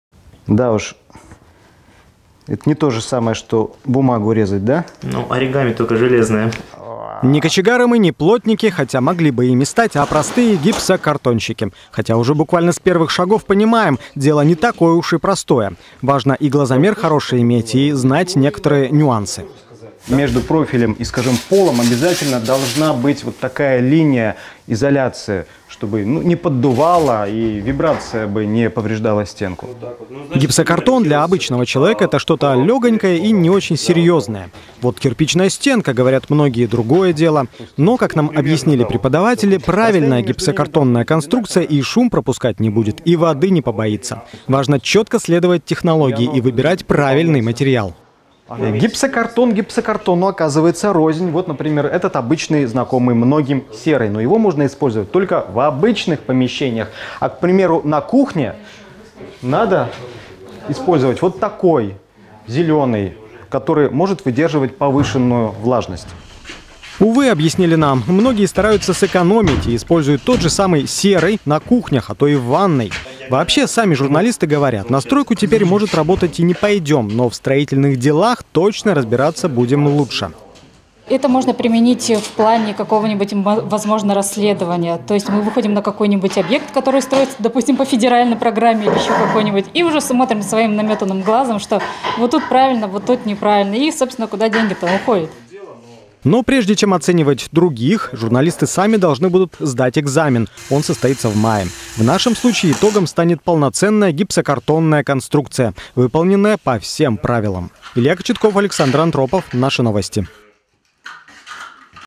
Как журналисты барнаульских СМИ постигают азы сухого строительства
В разгаре – очередной «сезон» ставшего популярным конкурса «Журналист меняет профессию». Больше 60-ти работников прессы сейчас пробуют себя в качестве пожарных, кинологов, флористов, даже лесорубов.